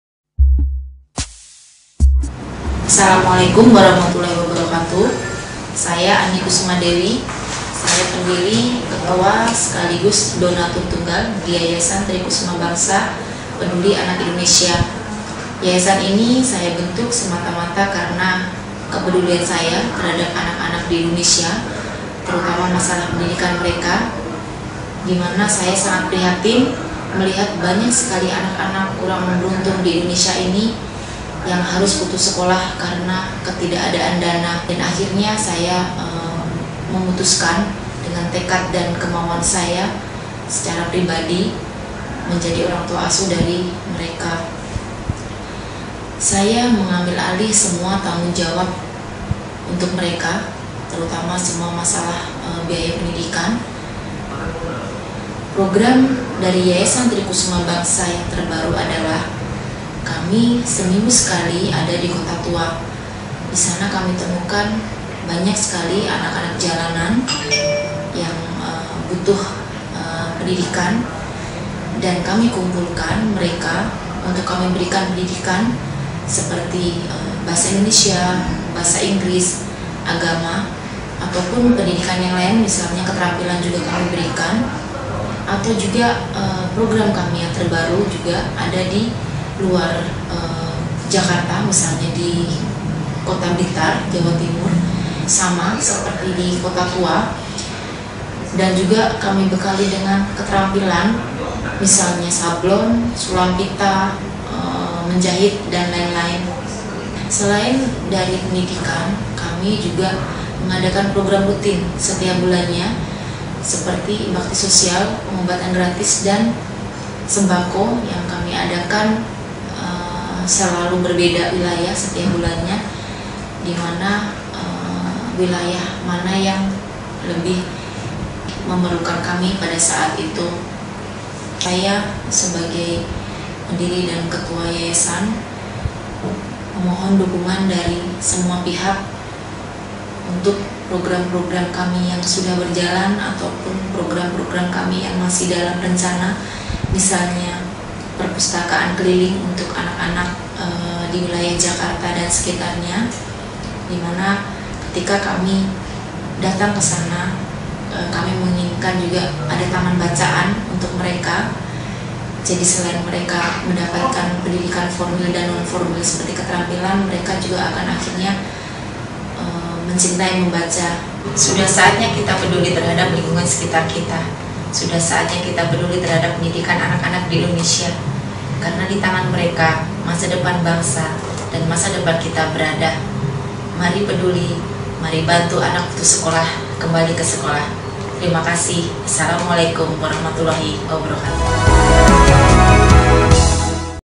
TRI KUSUMA BANGSA
Mari bantu anak putus sekolah kembali ke Sekolah